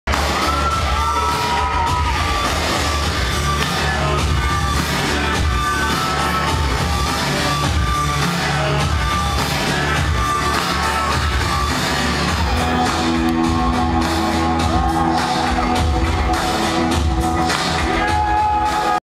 Snippet of Alan Wilder (Recoil) & Paul Kendall at "A Strange Hour," NYC (V)
Snippet of Alan Wilder (Recoil) & Paul Kendall at "A Strange Hour," NYC @ Le Poisson Rouge. Unbelievable visuals!